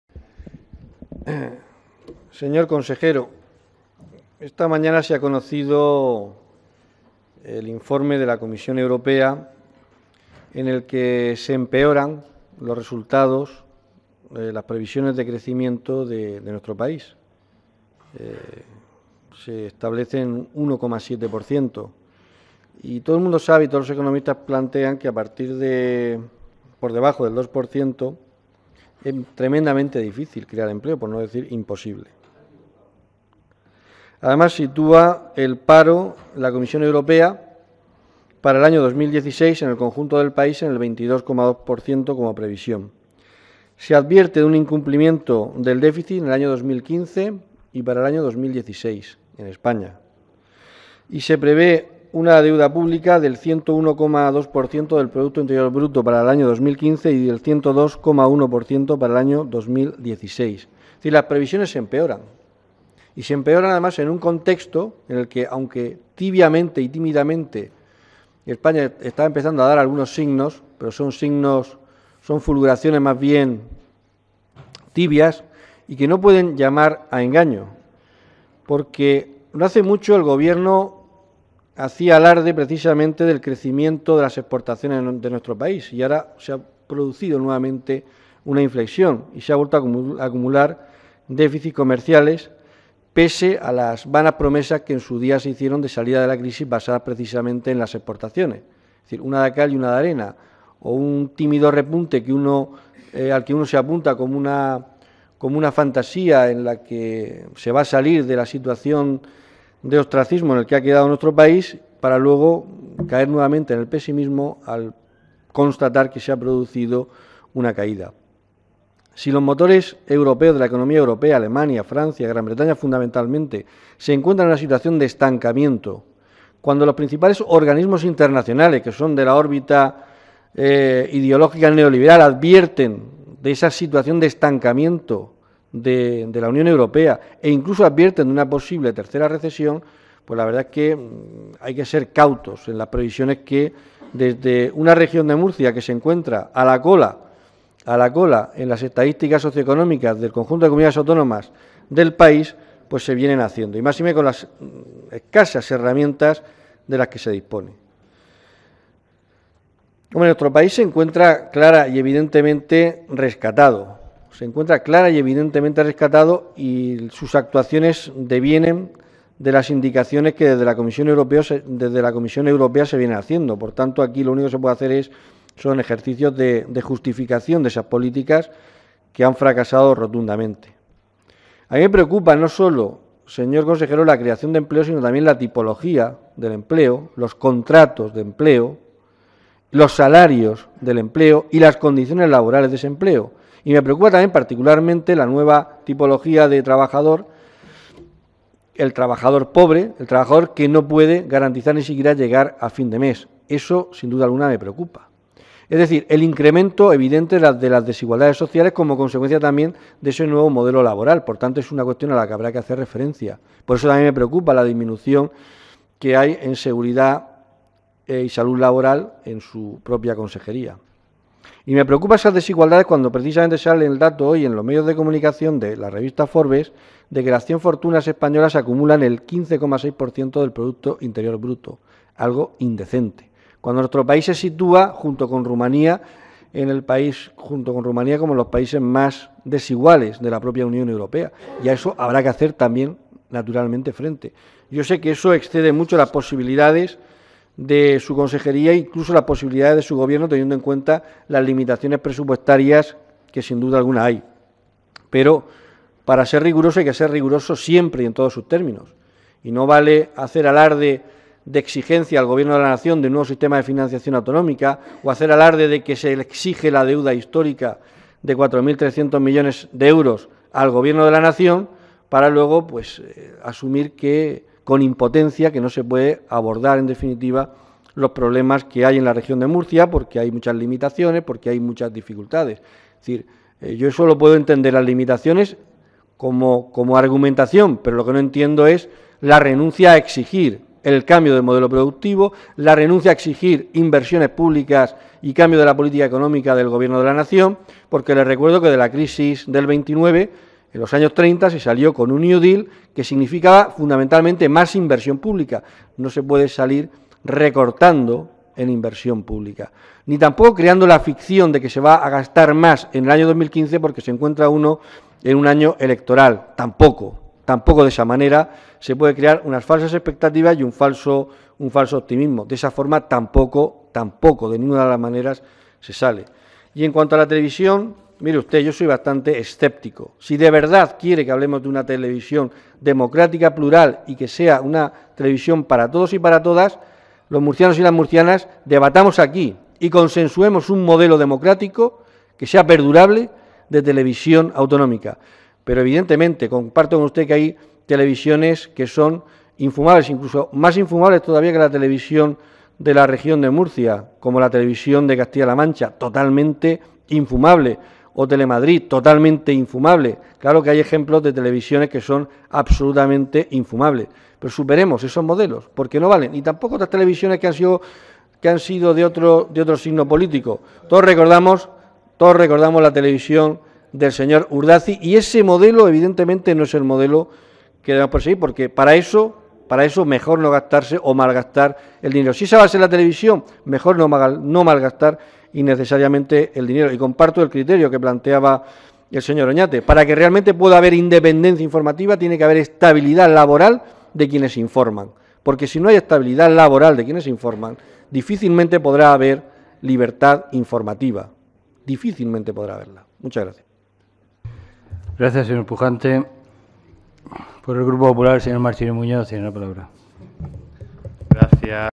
12 PUJANTE DIEKMANN JOSE ANTONIO
COMPARECENCIA DEL CONSEJERO DE PRESIDENCIA Y EMPLEO PARA INFORMAR SOBRE EL PROYECTO DE LEY DE PRESUPUESTOS GENERALES DE LA COMUNIDAD AUTÓNOMA PARA EL AÑO 2015, CORRESPONDIENTE A LA SECCIÓN 11, DEL ESTADO DE GASTOS DEL CITADO PROYECTO DE LEY.